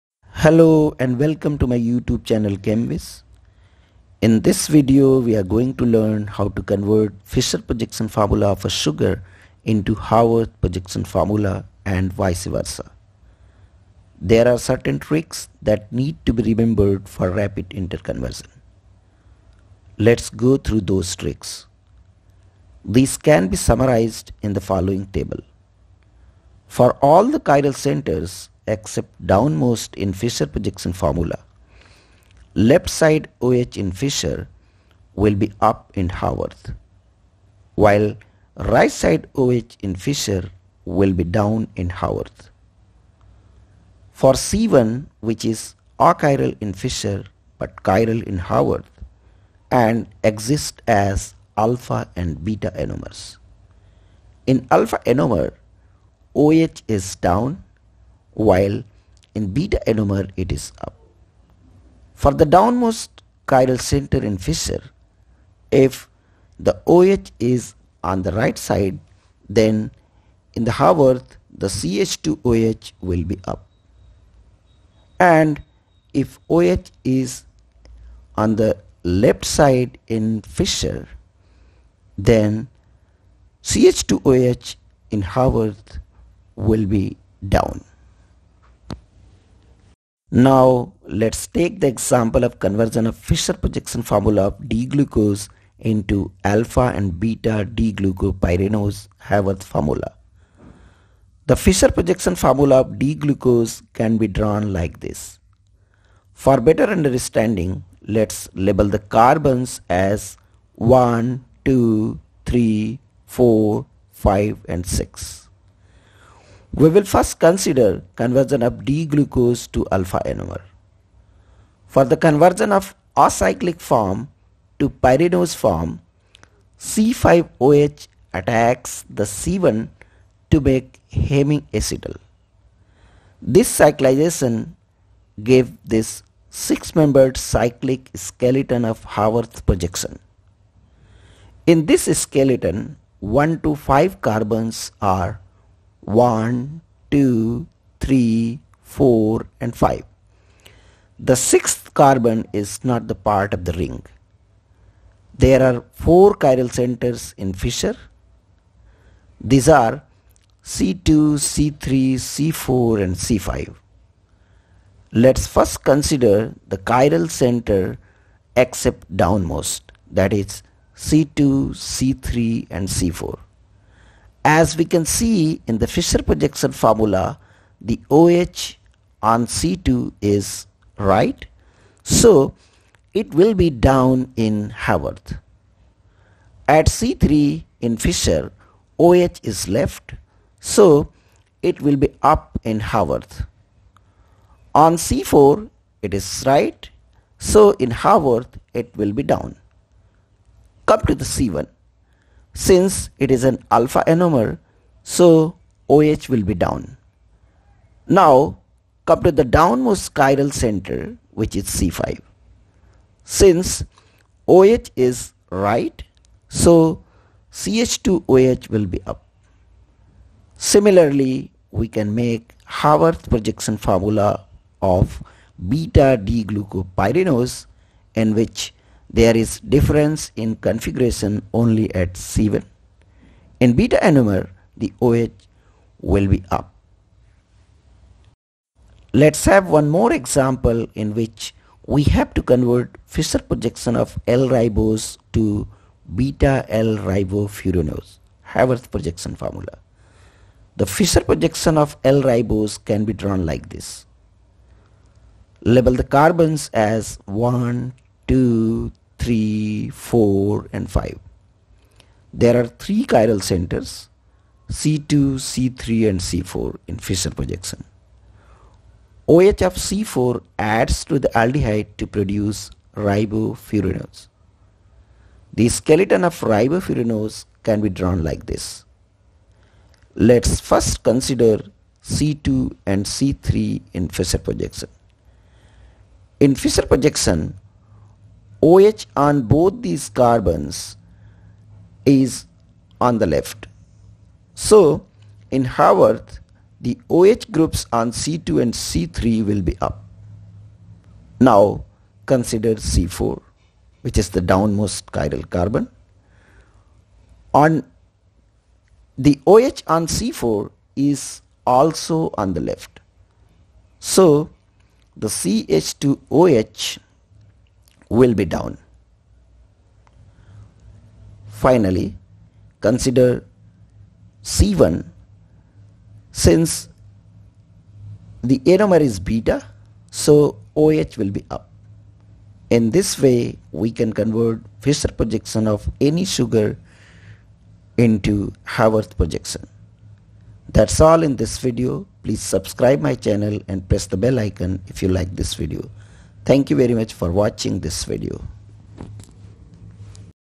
Tricks for the inter-conversion of Fischer projection to Haworth projection
Projection formulas are different ways of representing a molecule. Acylic form of sugar molecule is represented by Fischer projection while cyclic form by Haworth projection formula. Conversion of one projection formula to other is a tricky job. In this video tricks for the inter-conversion of Fischer to Haworth and vice versa have been discussed in detail with appropriate examples.